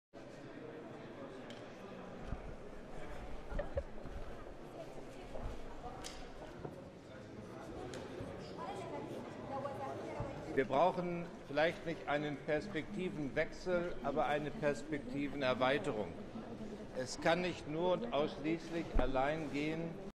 Germany: Steinmeier, Salam stress danger of Syrian refugee issue
M/S Rashid Derbas, Tammam Salam, Frank-Walter Steinmeier and Gebran Basil

C/U Press

SOT, Frank-Walter Steinmeier, German Foreign Minister (German): "We need an extended perspective rather than a change of perspective. We cannot solely deal with providing humanitarian aid to the refugees, but need to take care of the stability of the host countries in particular."

C/U Press

SOT, Frank-Walter Steinmeier, German Foreign Minister (German): "Together with representatives from more than 35 states who gathered here today, we want to ensure that our concern is aimed at countries like Lebanon as well. We want to help the government, but in particular we want to help the host communities, securing their structures for the future and therewith relieve host countries' burden."

M/S Sign

SOT, Tammam Salam, Lebanese Prime Minister (Arabic): "Many challenges and obstacles face us, and one of those is the reason we are here today in Berlin, and that is the Syrian crisis and the responsibility and weight that the Lebanese state, and other states, carry."

C/U Sign

W/S German Foreign Ministry

SCRIPT

The Syrian refugee crisis must be dealt with by ensuring the stability of major refugee host countries, German Foreign Minister Frank-Walter Steinmeier said at the Conference on the Syrian Refugee Situation in Berlin on Tuesday.

Steinmeier added that help must not only be provided to governments but also to host communities who help deal with the influx of refugees. Lebanese Prime Minister Tammam Salam continued that there are many challenges faced by Syria's neighbouring countries, the refugees issue being one of them.

Steinmeier is co-chair of the conference at the German Foreign Ministry alongside German Minister for Economic Cooperation and Development Gerd Mueller and the UN High Commissioner for Refugees Antonio Guterres.